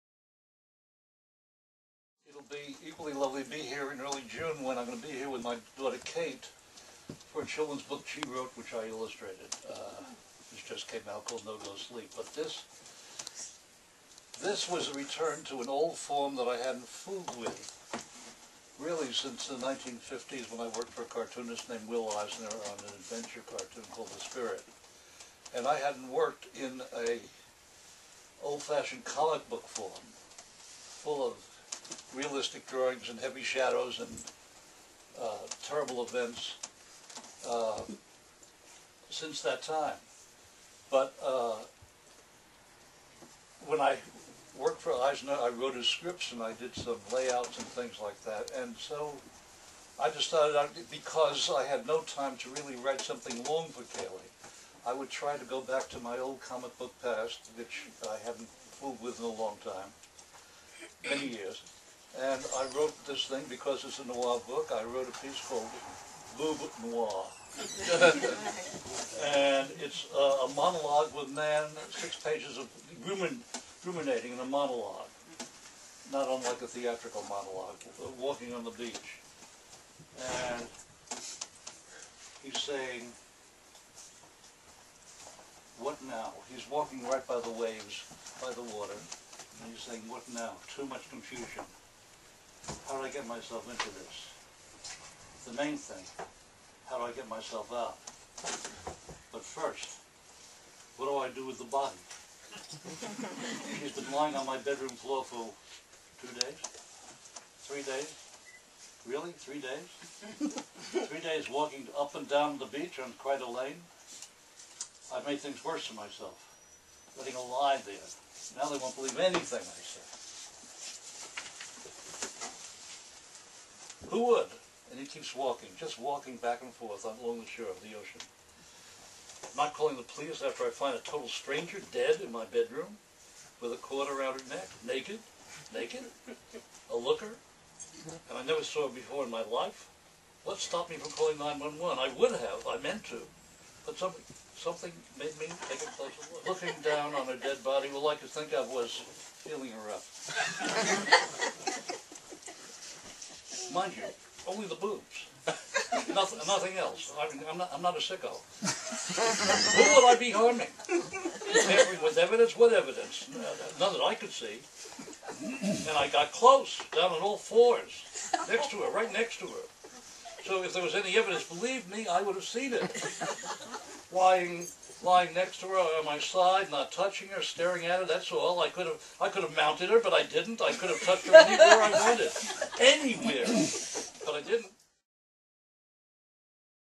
Jules Feiffer reads from "Boob Noir"
Pulitzer Prize-winning author and cartoonist Jules Feiffer reads from his comic strip "Boob Noir," which appears in Akashic Books' anthology series "Long Island Noir." In this comic strip, a man walks along the beach ruminating about a dead woman in his room. As he walks along the shore, the reader becomes more enlightened than the character.